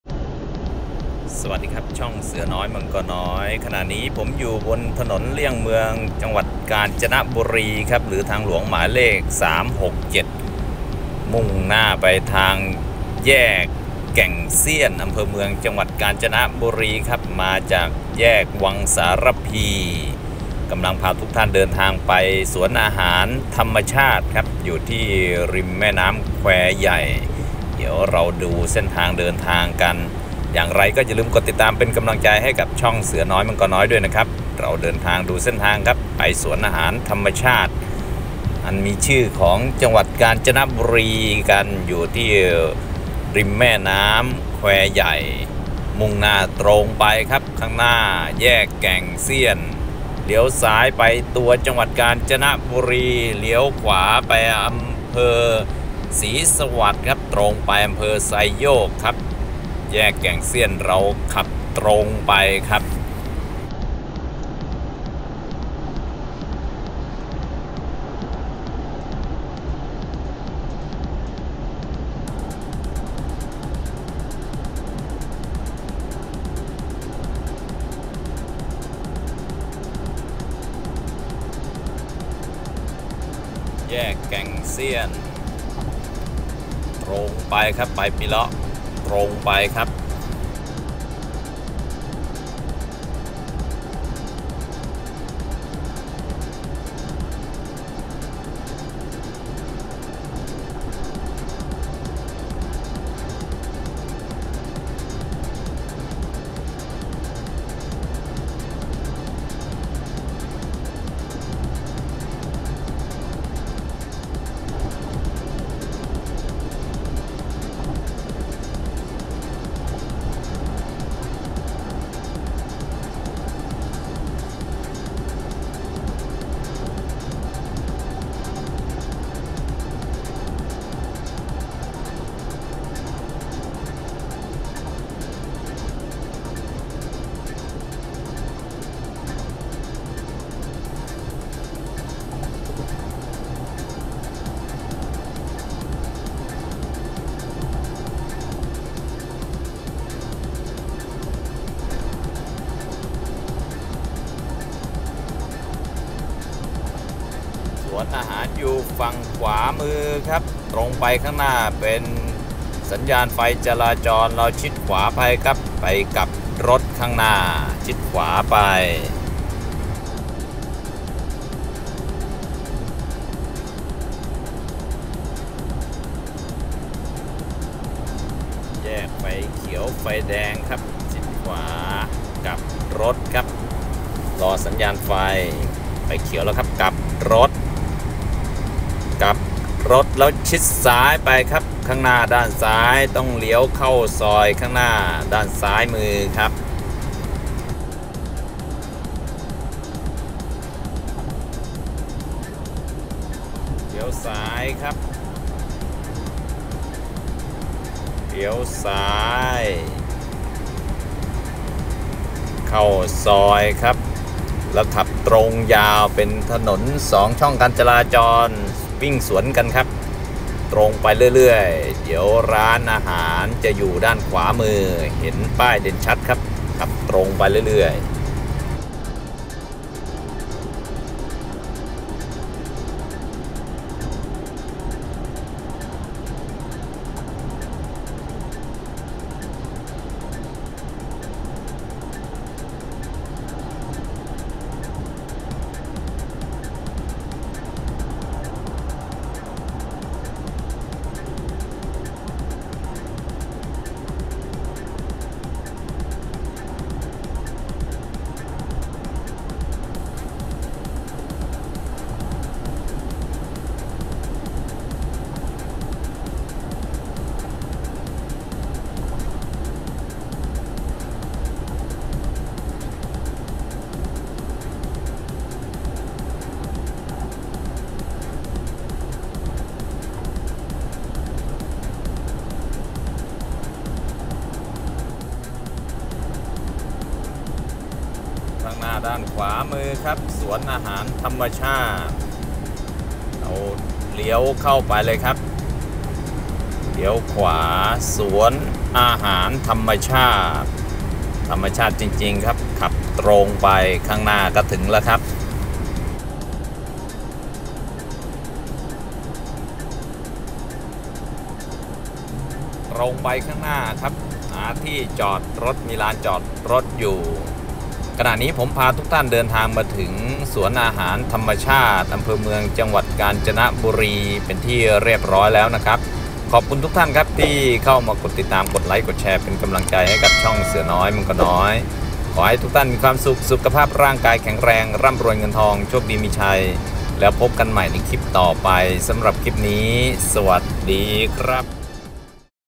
เส้นทางไปร้านอาหารธรรมชาติ ริมแม่น้ำแควใหญ่ จ.กาญจนบุรี
การเดินทางจากแยกแก่งเสี้ยน อำเภอเมือง จังหวัดกาญจนบุรี ไปสวนอาหารธรรมชาติ ริมแม่น้ำแควใหญ่ อำเภอเมือง จังหวัดกาญจนบุรี ￼